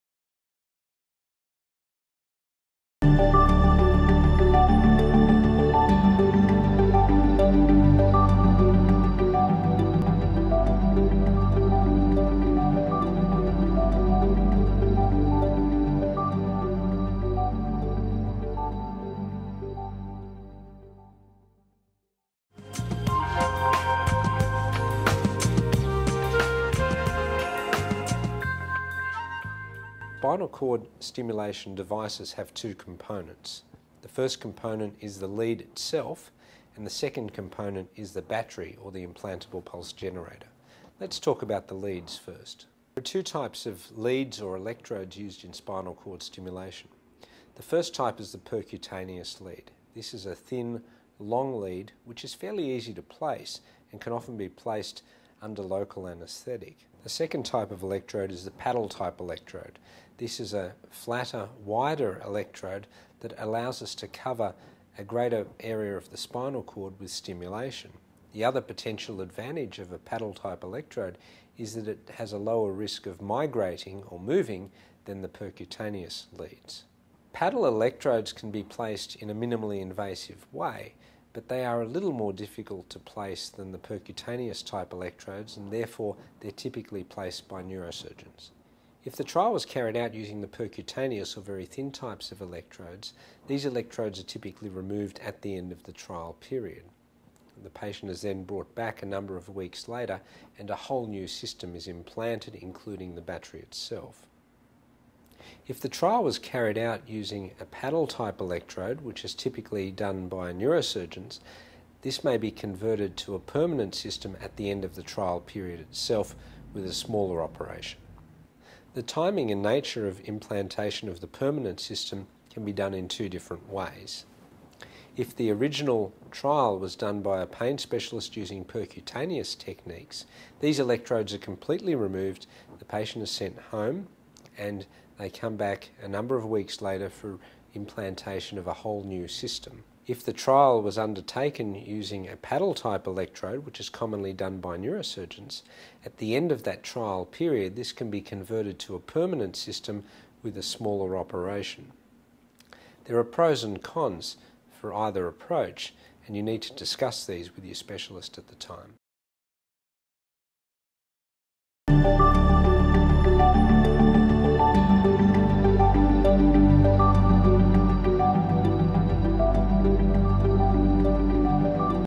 Spinal Cord Stimulation Lead types and placements Patient and GP Education
Spinal Cord Stimulation   Lead types and placements    Patient and GP

WHAT IS SPINAL CORD STIMULATION?

You have probably had many treatments for your pain, most of which will have been of little or no long-term help. Spinal cord stimulation works by targeting the region of the spinal cord involved in the transmission and processing of pain. We introduce a small amount of electrical current to the back of the spinal cord, which helps to mask the pain that you are feeling. It also increases the blood flow to the heart and legs, often helping patients with angina and peripheral vascular disease.

WHO MIGHT BE SUITABLE FOR SPINAL CORD STIMULATION?

A number of painful conditions can be treated using spinal cord stimulation.
Patients with the following conditions who have failed all standard medical therapies may benefit:
Previous spinal surgery with ongoing leg pain
Complex regional pain syndromes (neuropathic pain, causalgia, reflex sympathetic dystrophy)
Nerve injuries (from trauma or previous surgery)
Angina (ischaemic heart disease)
Peripheral vascular disease